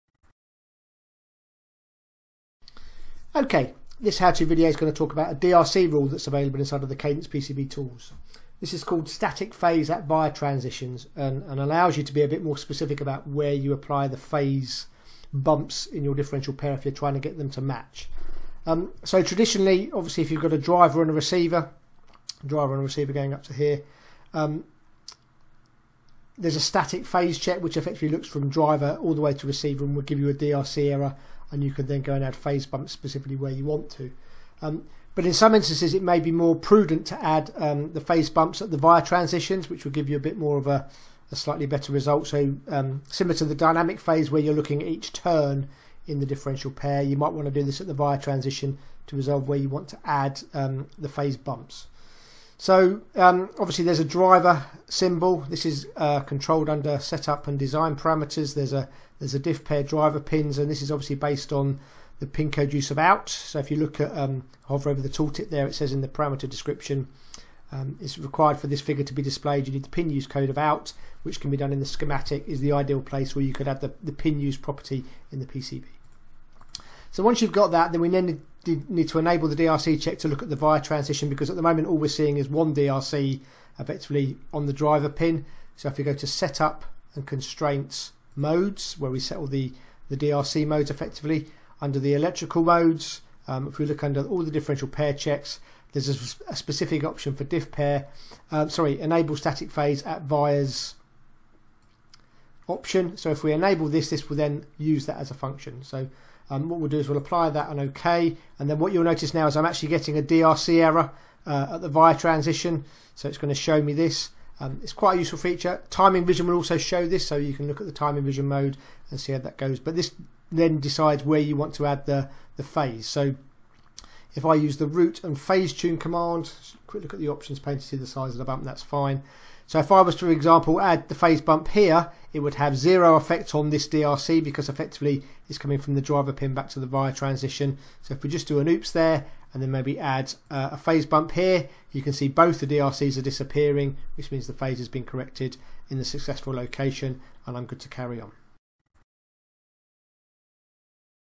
Allegro PCB High Speed Static Phase at Via Transitions
Here we explore the Allegro PCB High Speed Static Phase at Via Transitions features